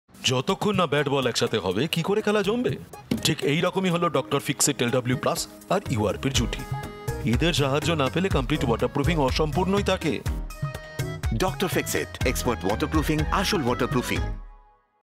Dr. Fixit | LW+ & URP Jodi Campaign | Bengali | 15 sec
Go for complete waterproofing of your new home during construction itself to avoid leakage and dampness later.
SMS DF to 57575 for Waterproofing Guide
OR